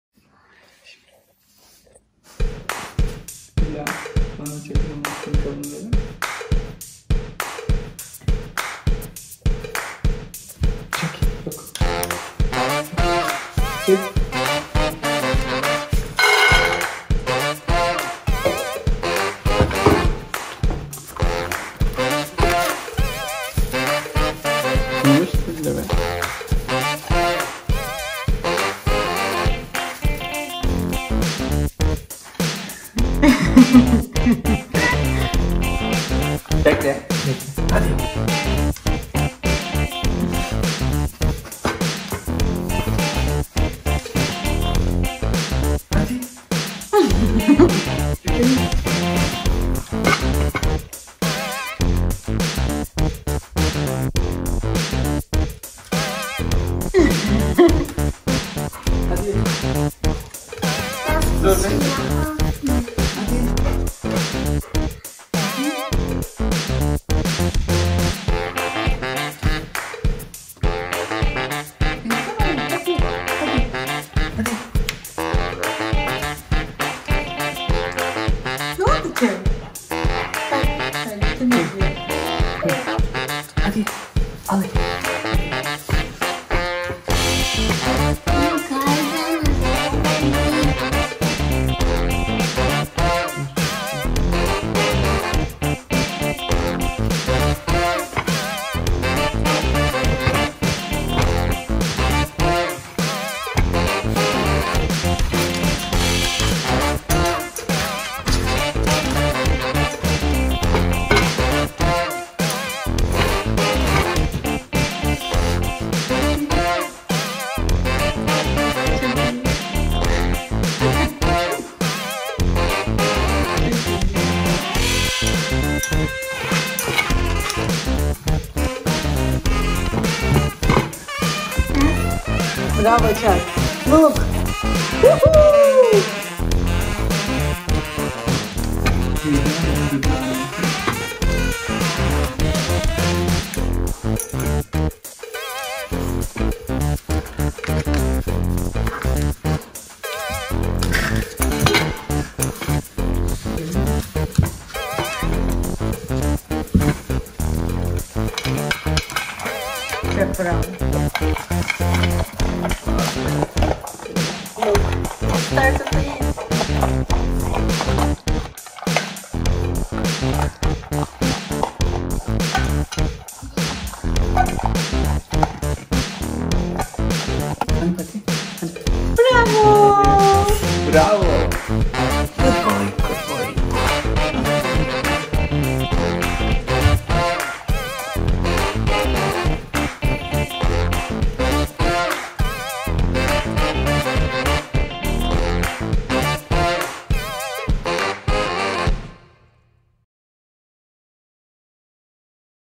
How Does My Labrador React When He Cannot Reach His Food | Chuck the Labrador Dog
Hi friends, this is the video of how my labrador reacts when he cannot reach his food. This is the Chuck the Labrador Dog's video. Chuck the Labrador is a funny and always hungry dog. He always wants to eat food. This time we made a surprise and we put his food into the bowl and cover with the folio. His reactions were super funny :) Please watch how he reacted :)

Please stay healthy and watch these funny labrador retriever dog videos.